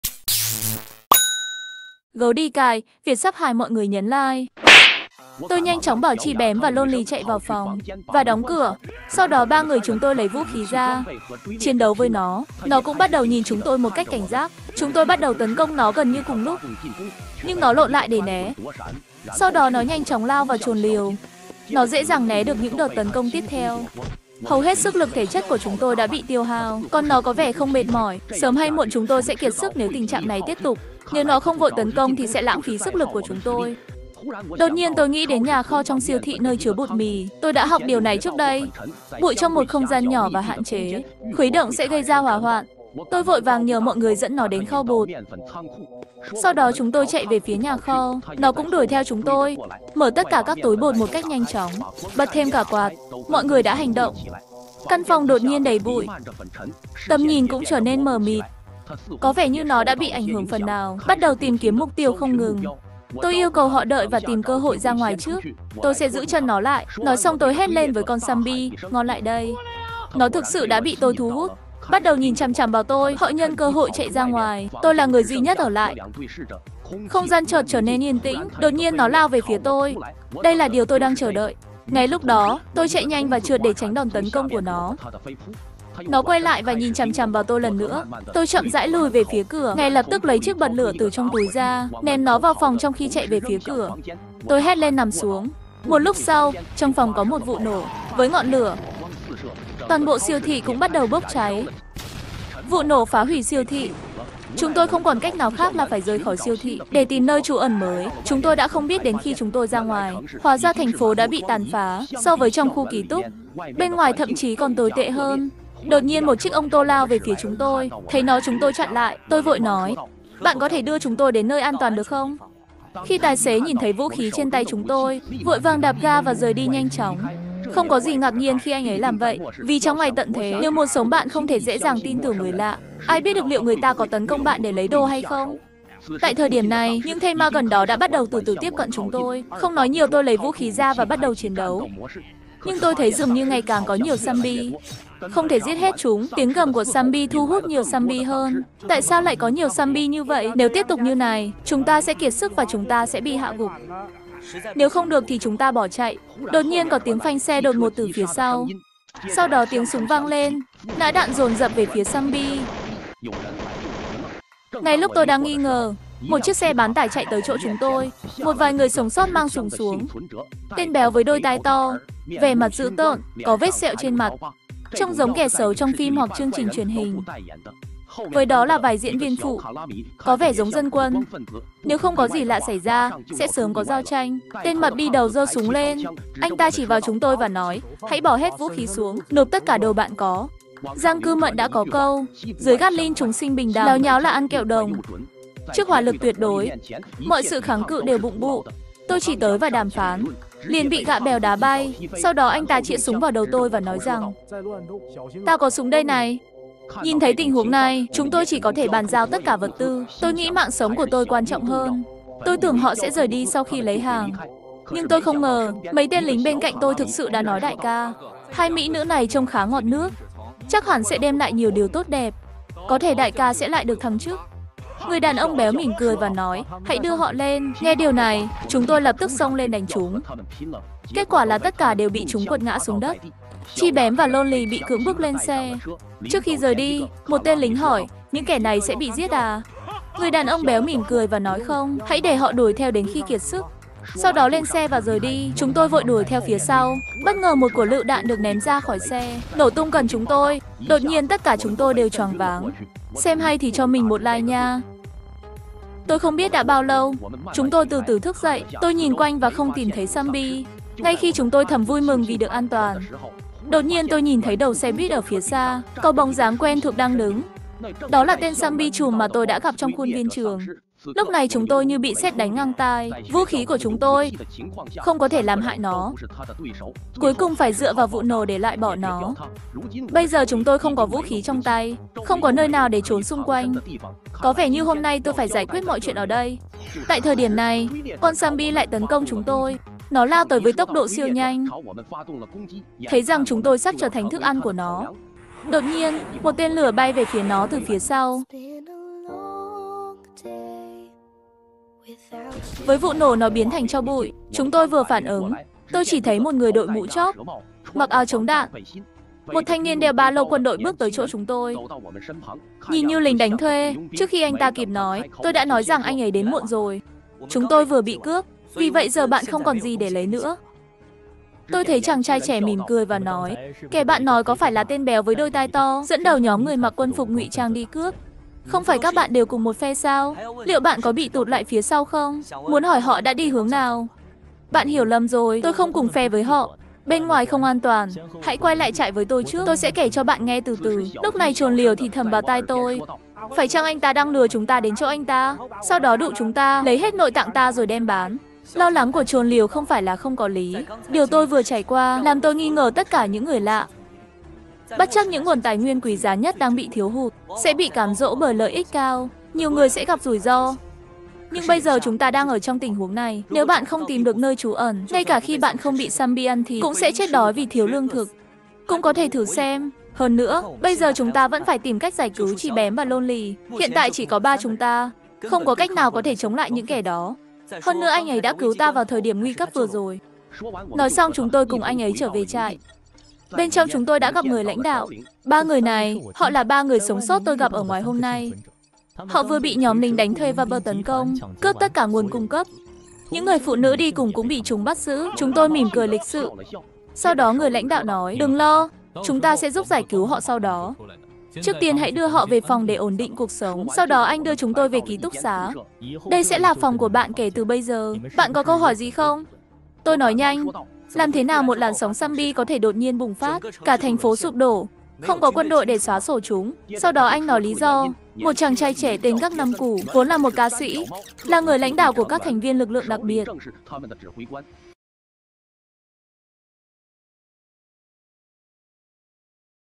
Ký túc xá Zombie - Tập 04 | Gấu Đi Cài
Ngày 1 tháng 1 năm 2033 gần tới Tết nên trường chúng tôi đã cho nghỉ học sớm, mọi người đa số đã về nhà, ký túc xá chỉ còn có tôi và Đồn Lầu ở lại. Sau 1 đêm hoan lạc chúng tôi bỗng bị đánh thức sớm bởi những âm thanh lạ lùng, đang định hỏi thăm sức khỏe phụ huynh vài người thì chúng tôi phát hiện hình như khu chúng tôi đã bị zombie xâm chiếm... tránh vỏ dưa gặp vỏ dừa chúng tôi bị quây bởi 1 lũ bặm trợn bị cướp vũ khí cả Chi Bém và Lôn Lỳ cũng bị bắt đi...